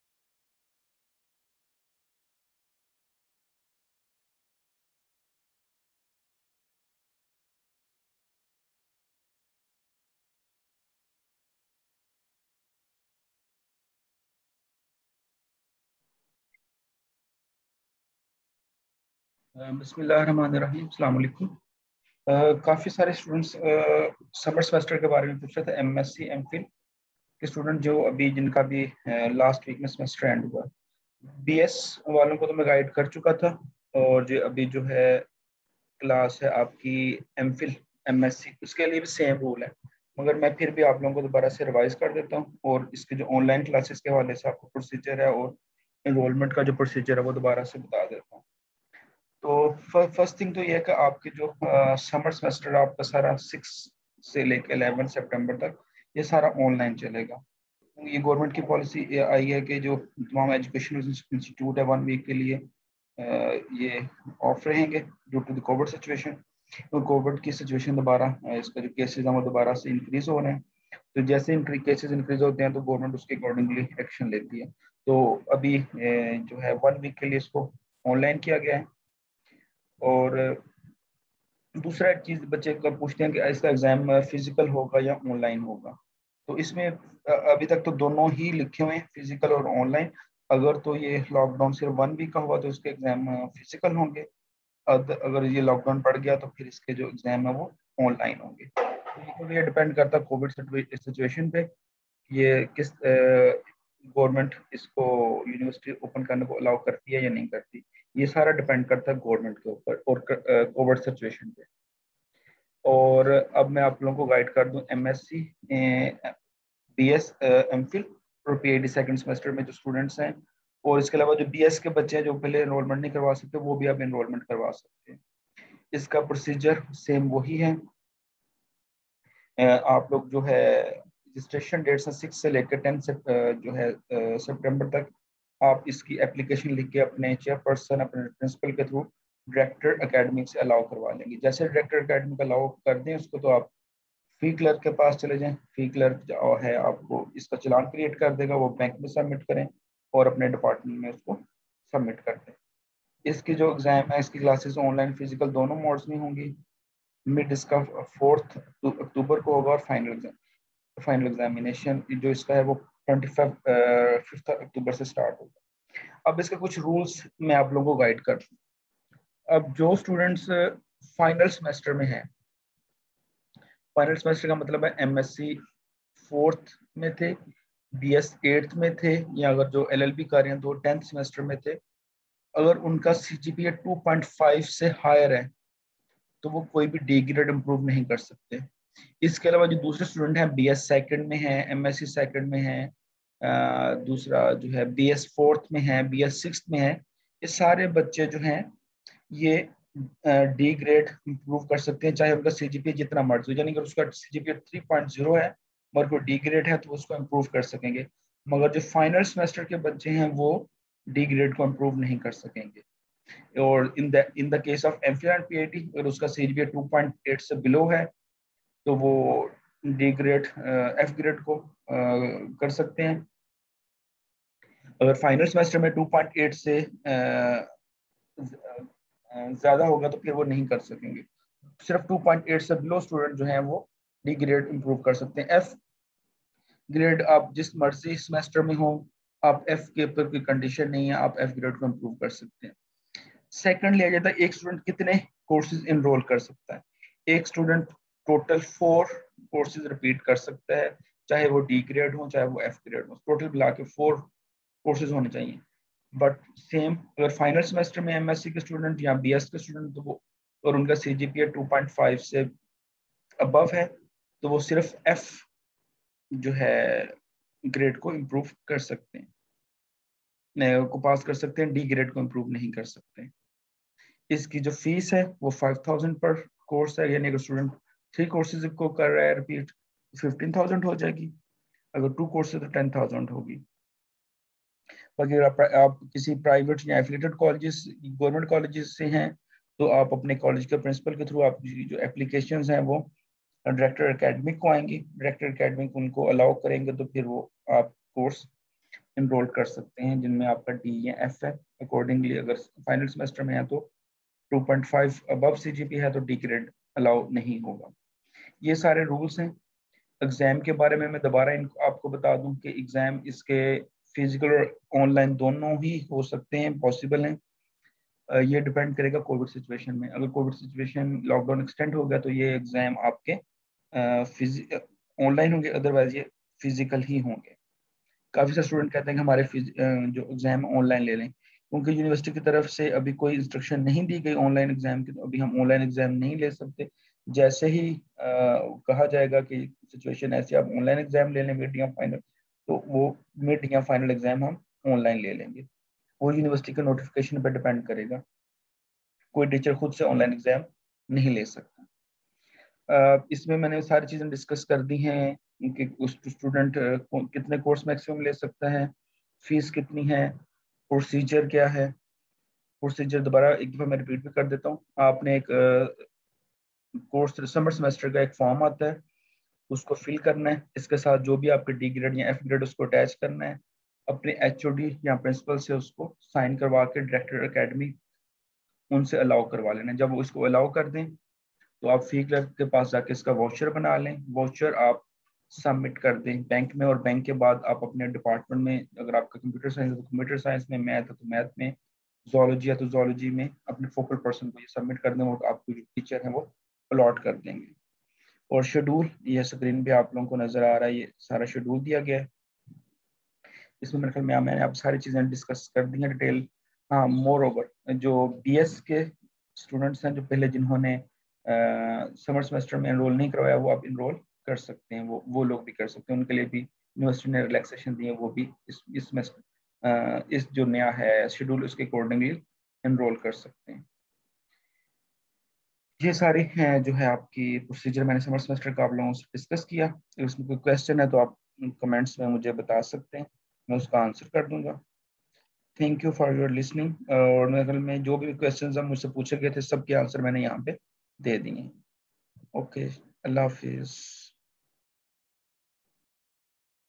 Summer Semester GCUF enrolment procedure and answers to students questions
University is open summer semester enrolment process